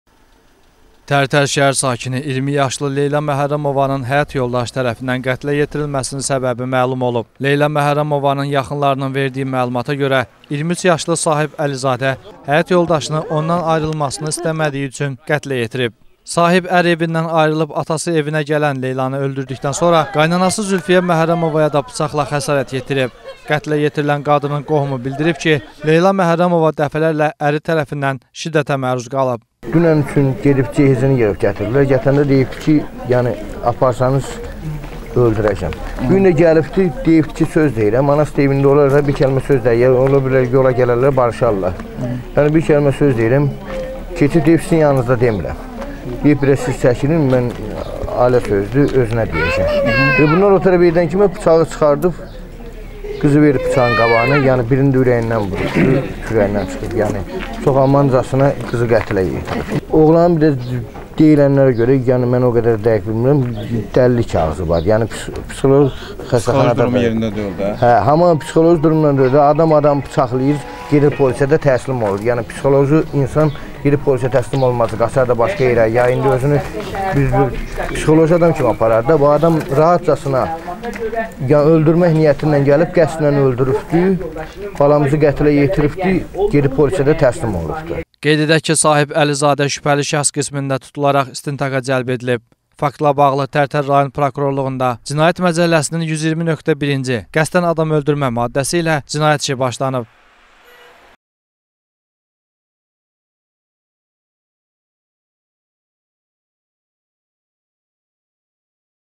20 yaşlı arvadını boşanmaq istəmədiyi üçün öldürdü – Tərtərdə dəhşətli qətlin təfərrüatı - APA TV
20 yaşlı arvadını boşanmaq istəmədiyi üçün öldürdü – Tərtərdə dəhşətli qətlin təfərrüatı - APA TV

Tərtər şəhər sakini 20 yaşlı Leyla Məhərrəmovanın həyat yoldaşı tərəfindən qətlə yetirilməsinin səbəbi məlum olub. Leyla Məhərrəmovanın yaxınlarının verdiyi məlumata görə, 23 yaşlı Sahib Əlizadə həyat yoldaşını ondan ayrılmasını istəmədiyi üçün qətlə yetirib. Sahib ər evindən ayrılıb atası evinə gələn Leylanı öldürdükdən sonra qaynanası Zülfiyyə Məhərrəmovaya da bıçaqla xəsarət yetirib.
Qətlə yetirilən qadının qohumu bildirib ki, Leyla Məhərrəmova dəfələrlə əri tərəfindən şiddətə məruz qalıb:
Qeyd edək ki, Sahib Əlizadə şübhəli şəxs qismində tutularaq istintaqa cəlb edilib. Faktla bağlı Tərtər rayon Prokurorluğunda Cinayət Məcəlləsinin 120.1-ci (qəsdən adam öldürmə) maddəsi ilə cinayət işi başlanıb.